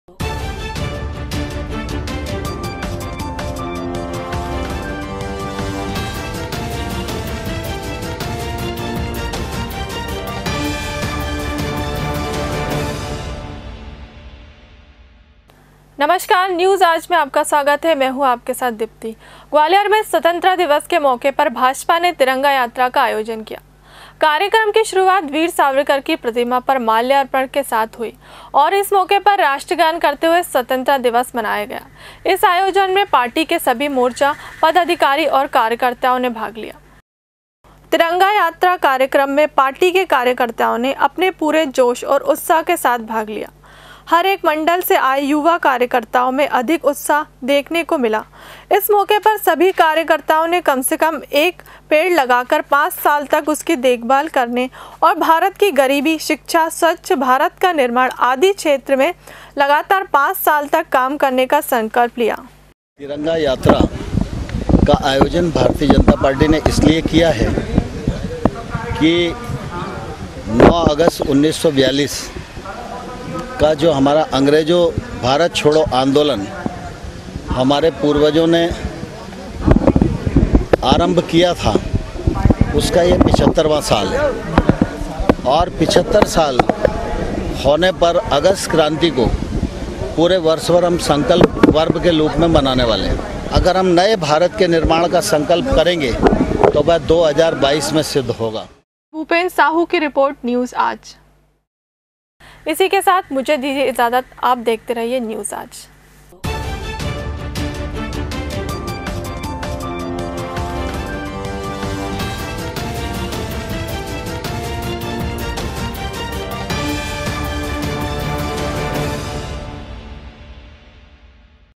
युवाओं का संकल्प हम करेंगे नए भारत का निर्माण "NEWS AAJ"
ग्वालियर में स्वतंत्रता दिवस के मौके पर भाजपा ने तिरंगा यात्रा का आयोजन किया